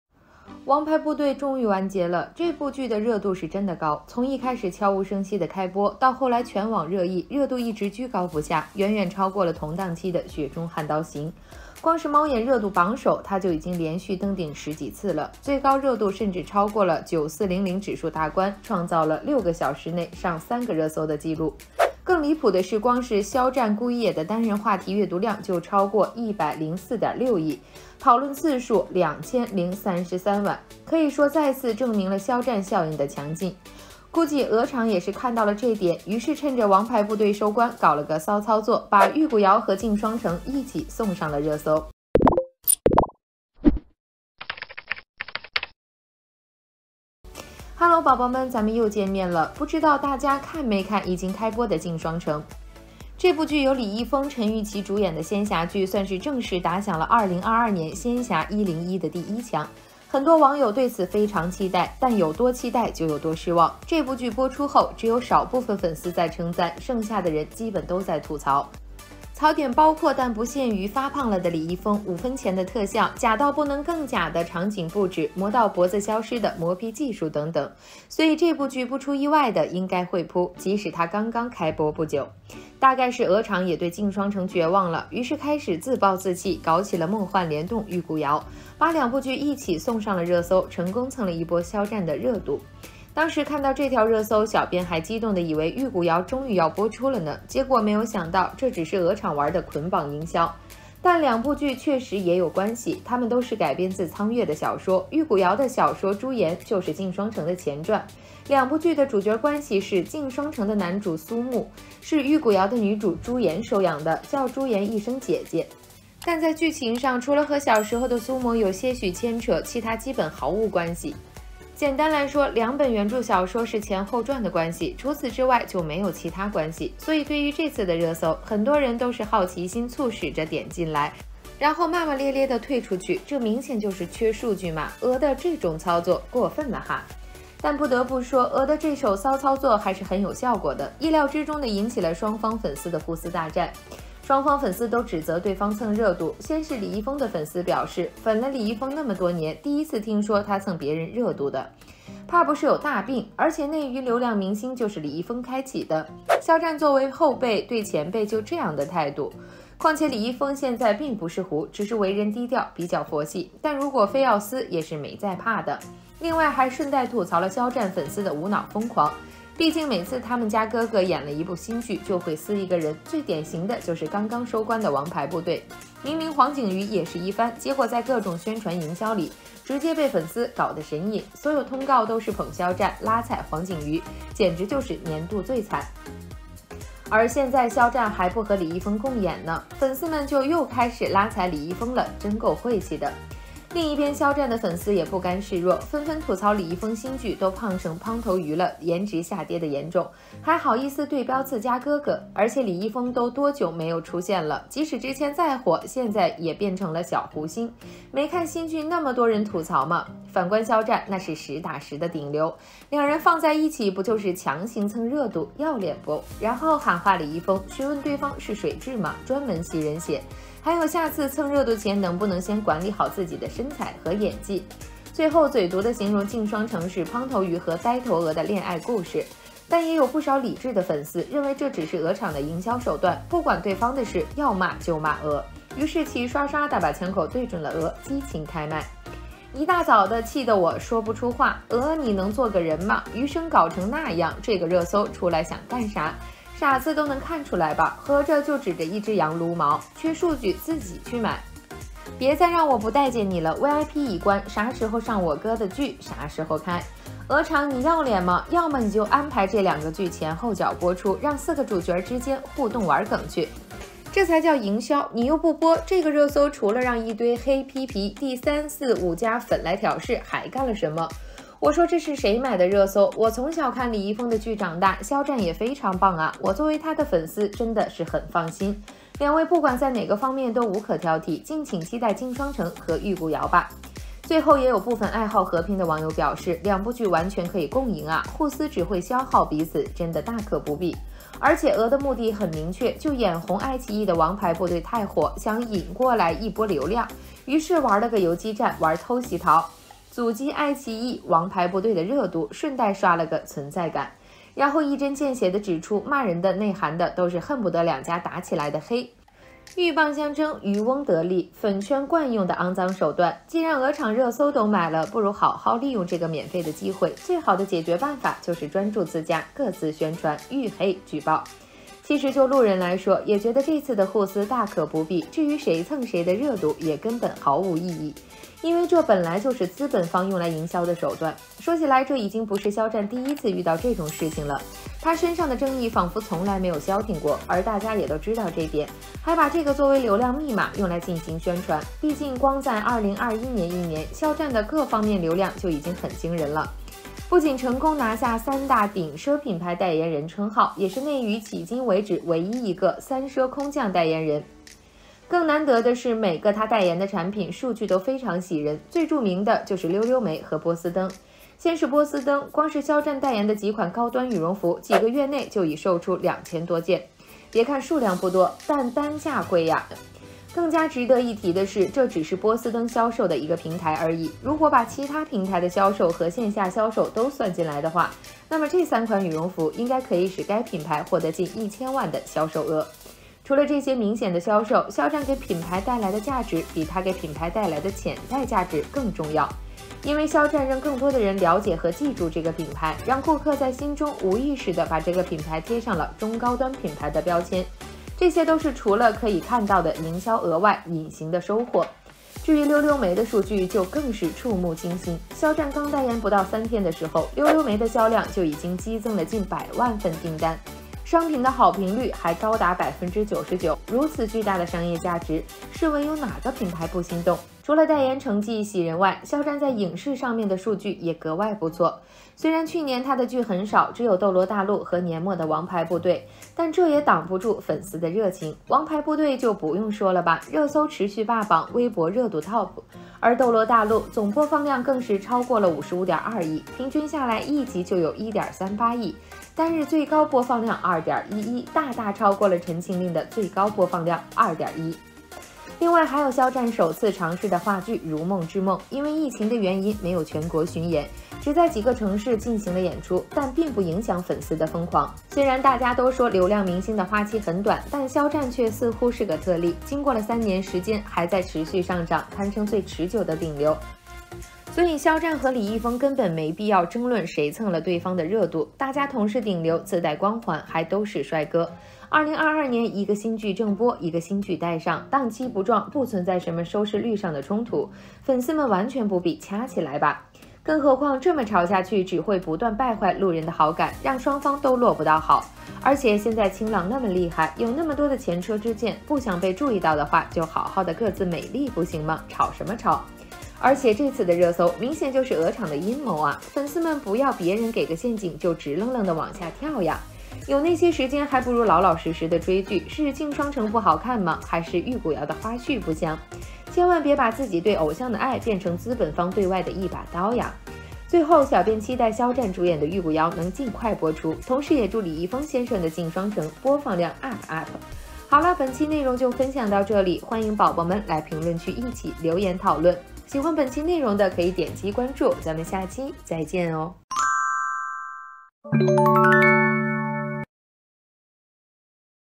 鵝廠宣發騷操作，肖戰再成背鍋俠！粉絲互相引戰！粉絲們！千萬不要把對偶像的愛，變成資本的一把刀啊！肖戰 李易峰 鏡雙城【紅楓娛樂】
《王牌部隊》終於完結了，這部劇的熱度是真的高。
從一開始消無聲息的開播，到後來全網熱議，熱度一直居高不下，遠遠超過了同檔期的《雪中悍刀行》。
光是貓眼熱度榜首，它就已經連續登頂十幾次了，最高熱度甚至超過了9400指數大關，創造了6個小時內上3個熱搜的記錄，更離譜的是，光是“肖戰顧一野”的單人話題閱讀量，就超過104.6億，討論次數2033萬。
可以說再次證明了“肖戰效應”的強勁。
估計鵝廠也是看到了這點，於是趁著《王牌部隊》收官，搞了個騷操作，把《玉骨遙》和《鏡雙城》一起送上了熱搜。